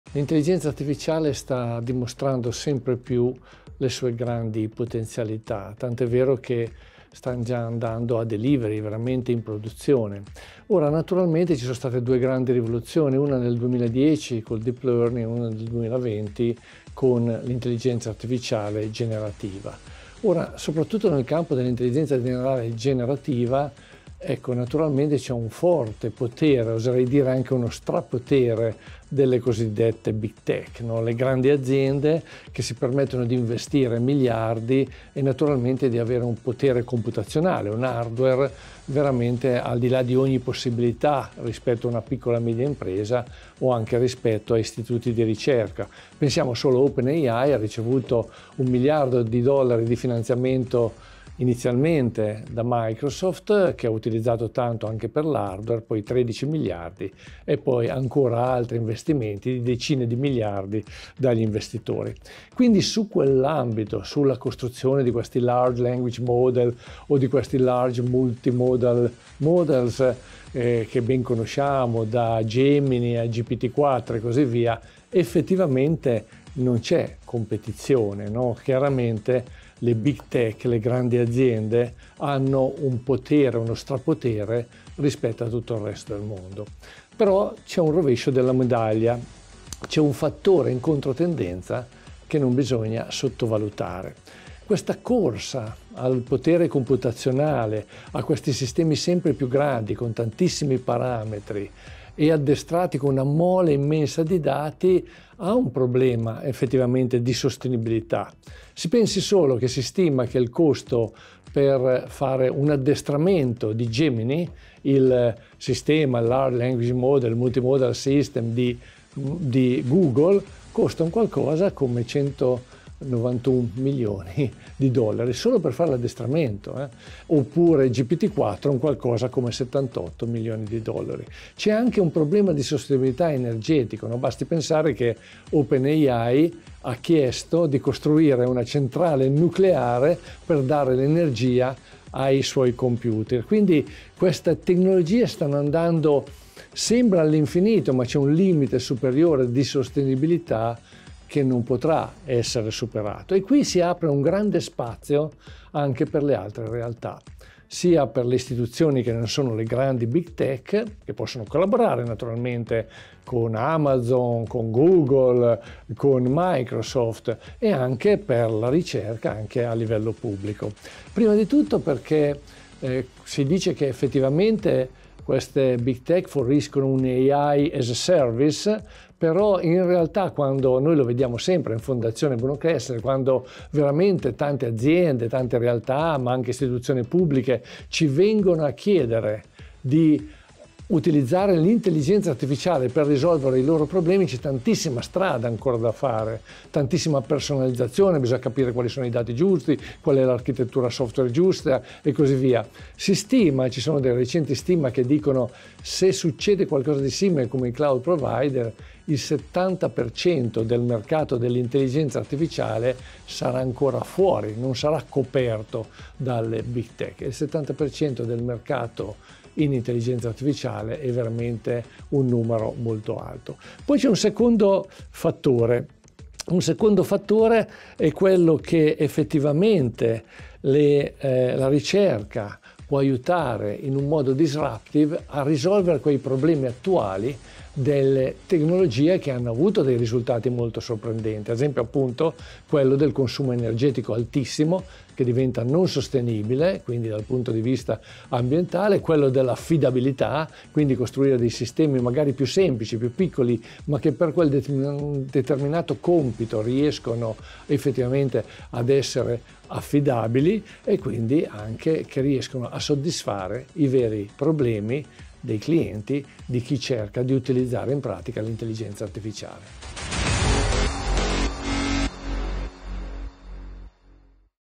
INTELLIGENZA ARTIFICIALE: LE STRATEGIE DEI BIG TECH
Paolo Traverso, direttore della Pianificazione Strategica della Fondazione Bruno Kessler